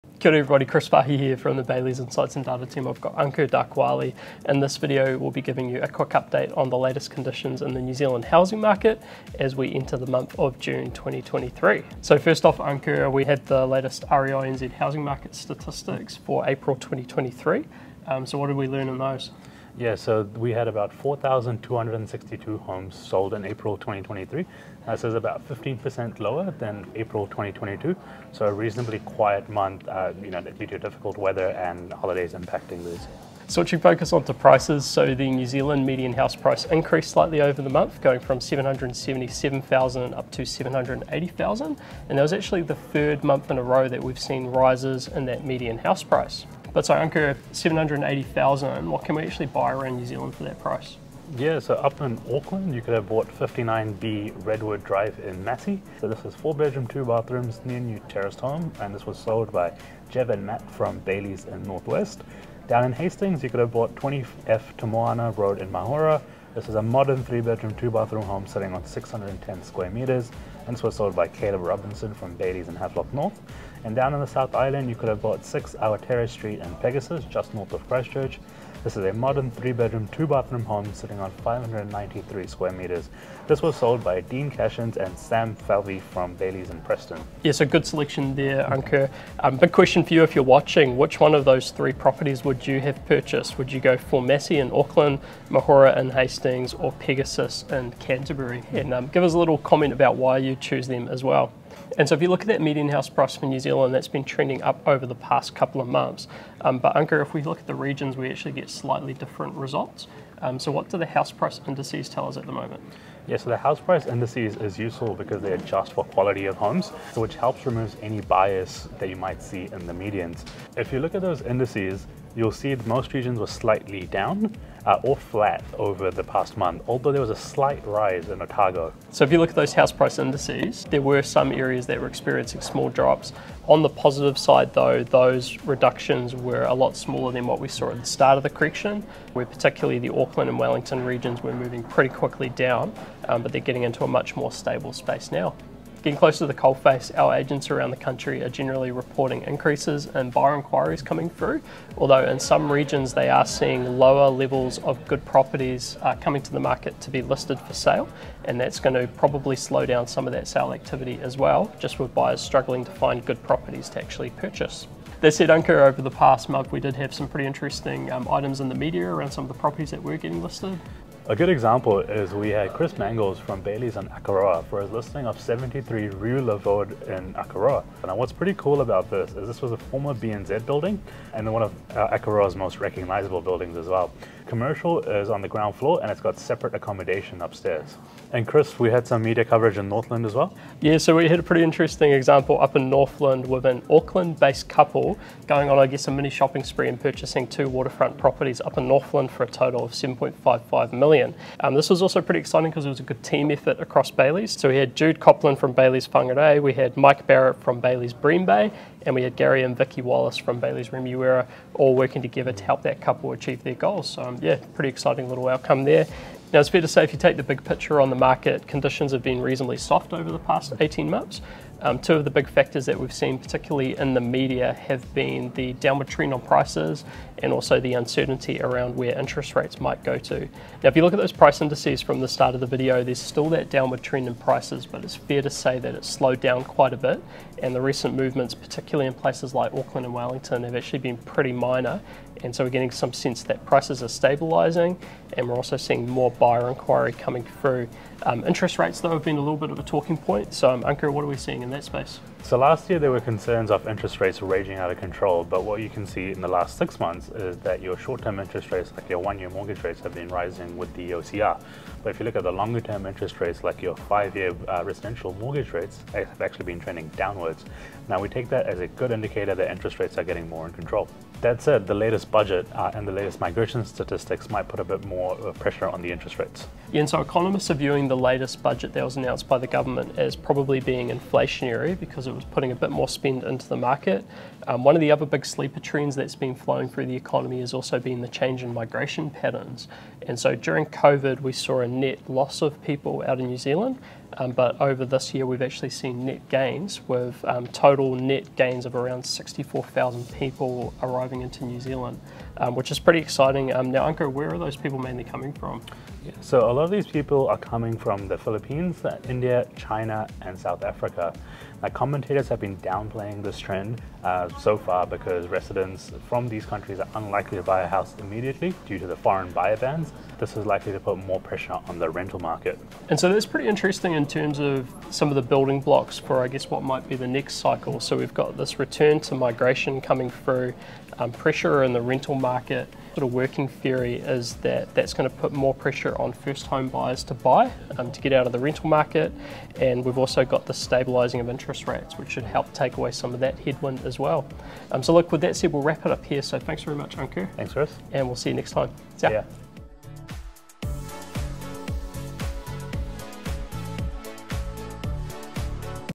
June 2023 - What’s happening with NZ house prices?
Chris Farhi and Ankur Dakwale from the Bayleys Insights & Data team cover the latest house price statistics, sample of recent sales, and the outlook for the housing market.

@bayleys real estate, @bayleys commercial

Bayleys Insights & Data (property market research and property consulting / advisory services)

Ankur Dakwale – Analyst – Insights, Data & Consulting